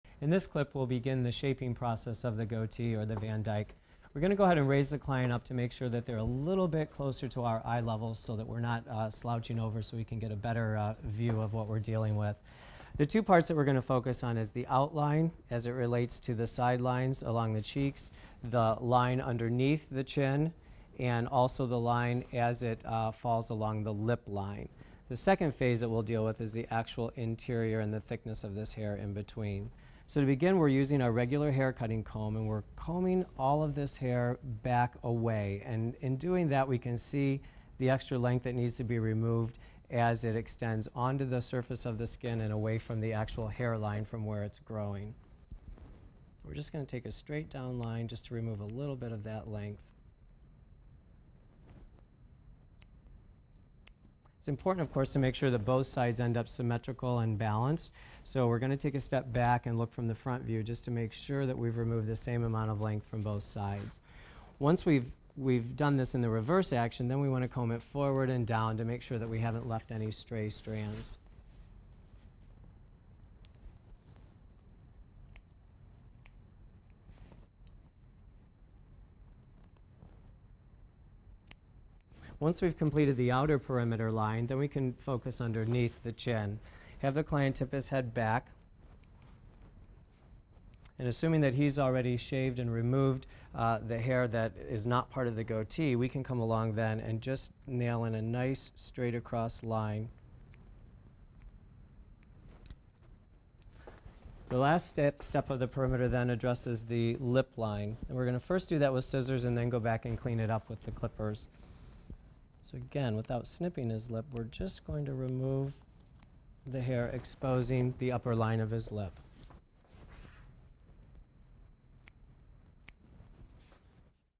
Van Dyke Goatee: Care & Coloring : Van Dyke Goatee: Trimming & Shaping Perimeter
Trimming and shaping the perimeter of a Van Dyke goatee requires maintaining symmetry on both sides of the facial hair and using straight, single cut lines with shears. Trim and shape the perimeter of a goatee with tips from a professional hair stylist in this free men's grooming video.

Expert: Kevin Charles
Bio: Kevin Charles is the owner of Kevin Charles Hair Artistry, a beauty salon, located in St. Petersburg, Fla.